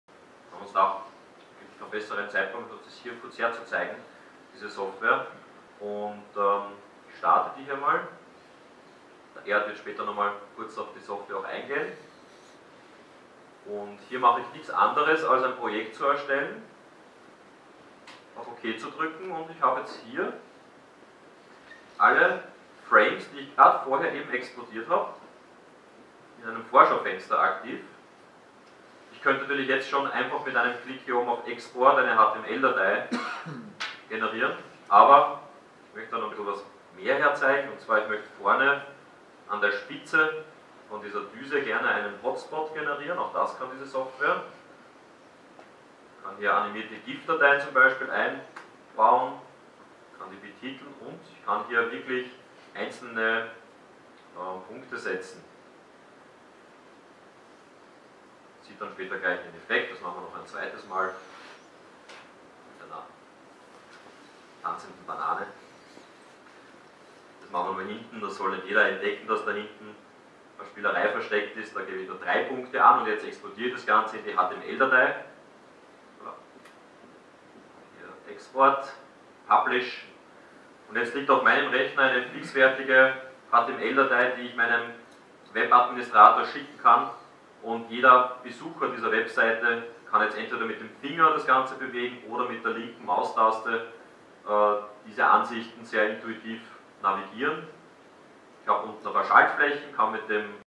To learn how to use SolidWorks with WebRotate 360 talk to guys at planetsoftware GmbH
For those of you who speaks German and is interested in 3D CAD, here's a video from one of the recent SolidWorks training events in Dornbirn, Austria by planetsoftware GmbH (a company of Bechtle AG) where the audience was given an introduction into an integration workflow with WebRotate 360 product viewing software.